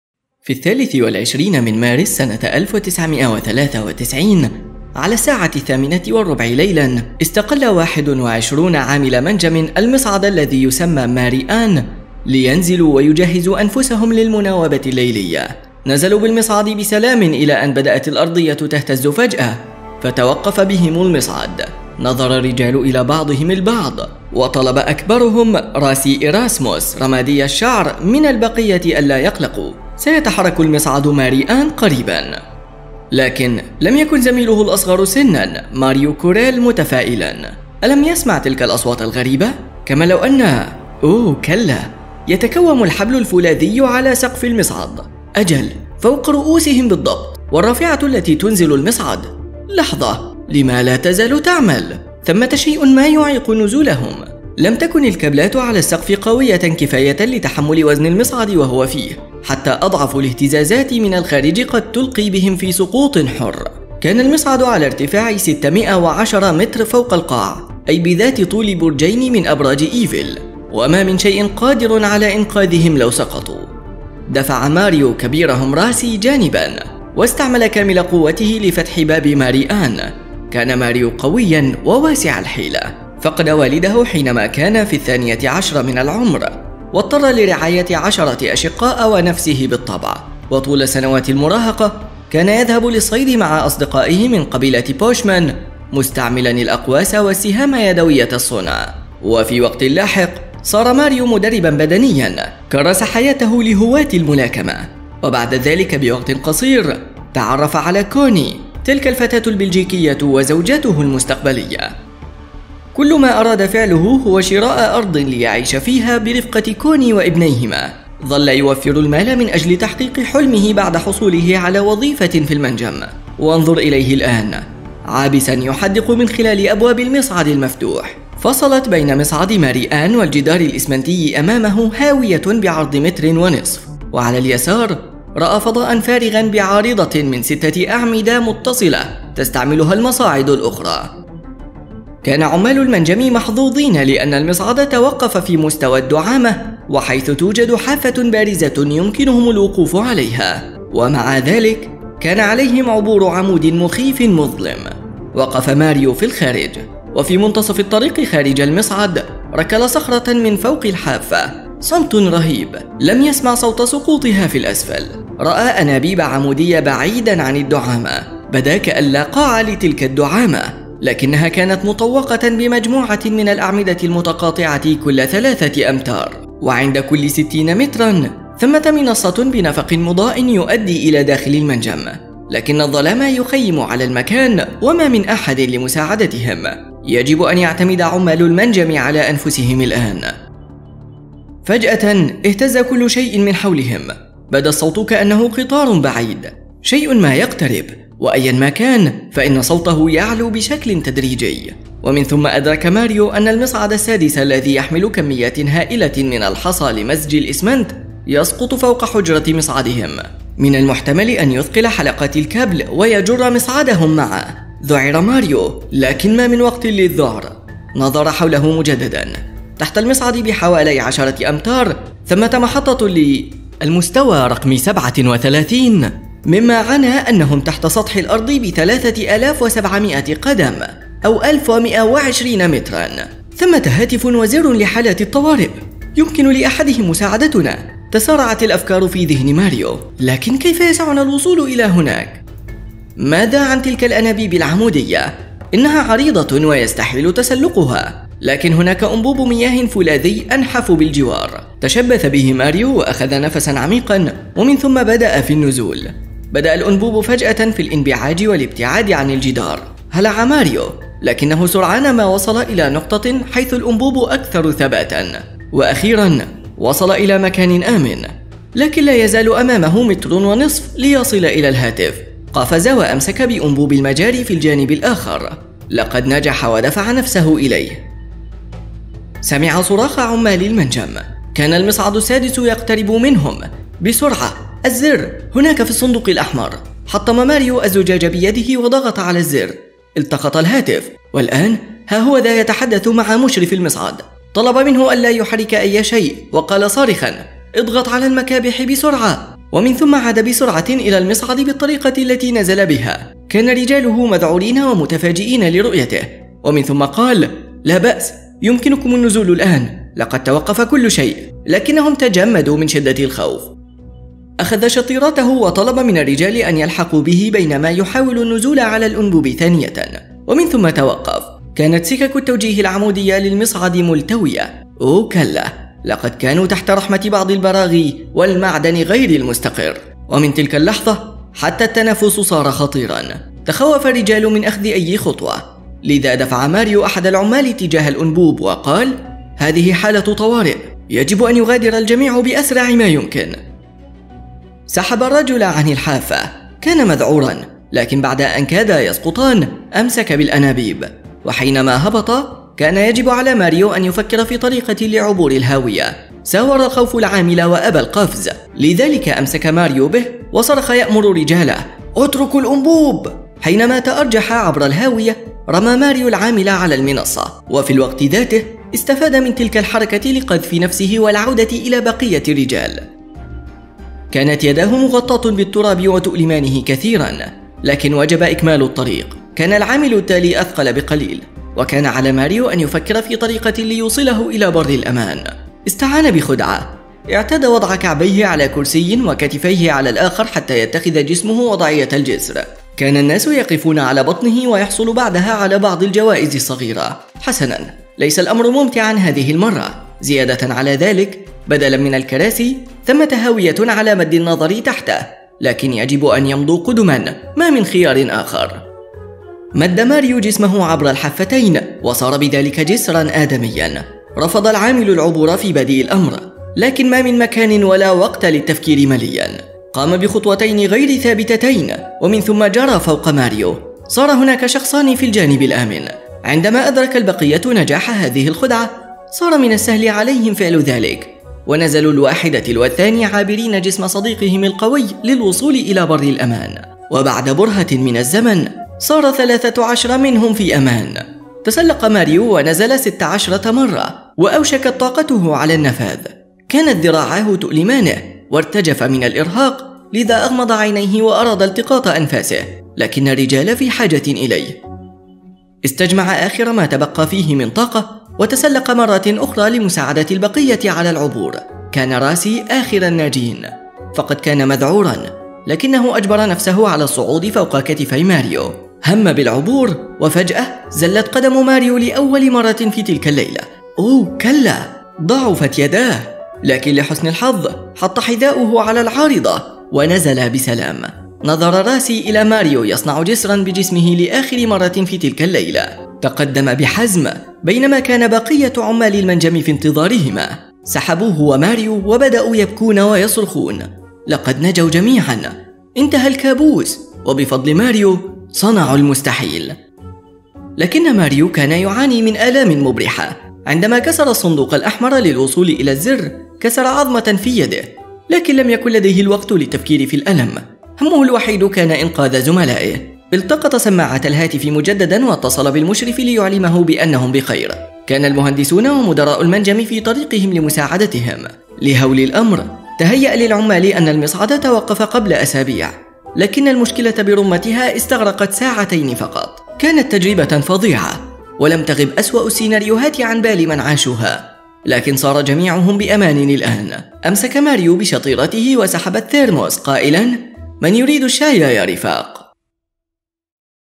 كيف أنقذ رجل واحد 20 عاملاً حوصروا في مصعد تحت الأرض
تخيل نفسك عالقا في مصعد... لكن ماذا عن مصعد مطمور تحت الأرض؟ 😱 هذه قصة واقعية: أنقذ رجل شجاع 20 شخصا عالق في برج مصعد المنجم. تنويه بسيط: صنع جسرا بجسمه! دائما ما تكون قصص الإنقاذ ملهمة للغاية. فهي تمنحنا الأمل وتعلّمنا بألا نستسلم قط! أصغ جيدا إلى هذه القصة المدهشة.

تم إنشاء الصور المتحركة من قبل الجانب المشرق.